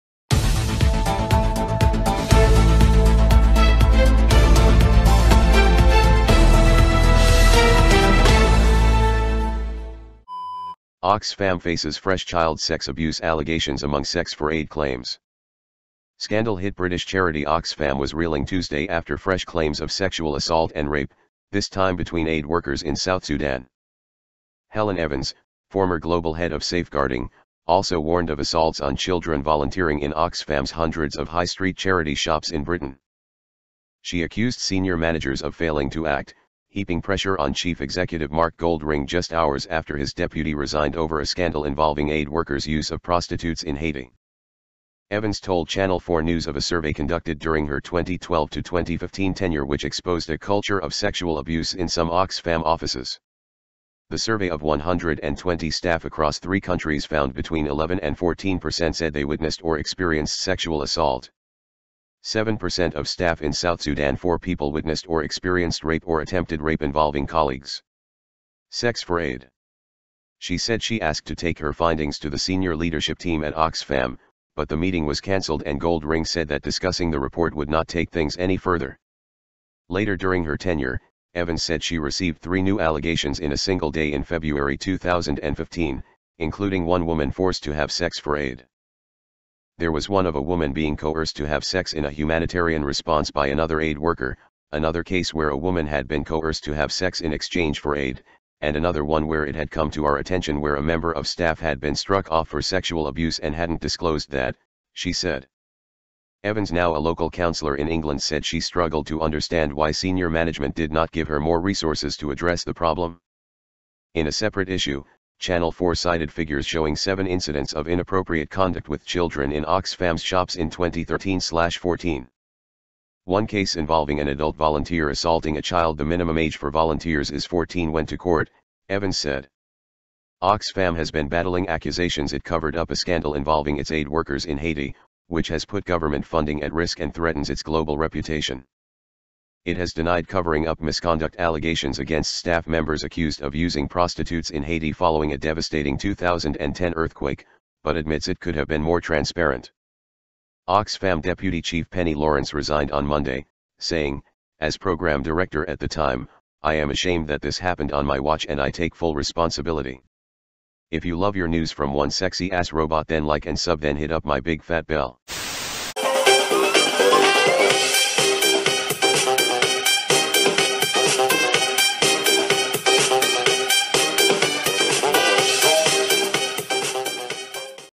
Oxfam Faces Fresh Child Sex Abuse, 'Sex For Aid' Accusations
Oxfam Faces Fresh Child Sex Abuse, 'Sex For Aid' Accusations Scandal-hit British charity Oxfam was reeling Tuesday after fresh claims of sexual assault and rape, this time between aid workers in South Sudan. Helen Evans, former global head of safeguarding, also warned of assaults on children volunteering in Oxfam’s hundreds of high-street charity shops in Britain.

She accused senior managers of failing to act, heaping pressure on chief executive Mark Goldring just hours after his deputy resigned over a scandal involving aid workers’ use of prostitutes in Haiti.